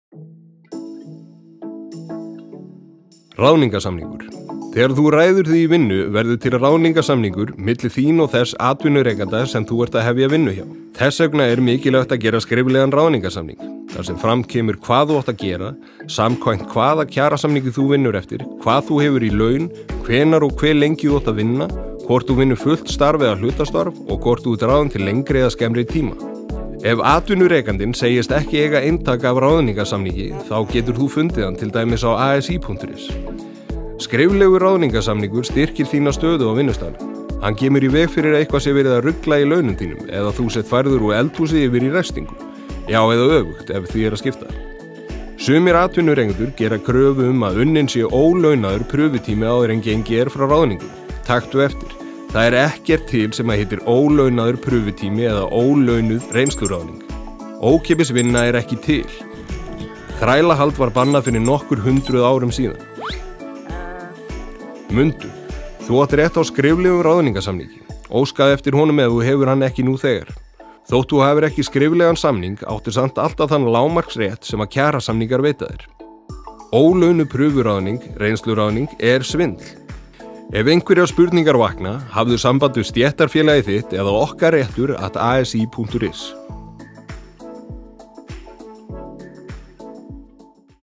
ASÍ - Ráðning
Fræðslumyndband fyrir ungt fólk um vinnumarkaðinn. Þetta fjallar um það að ráða sig í vinnu.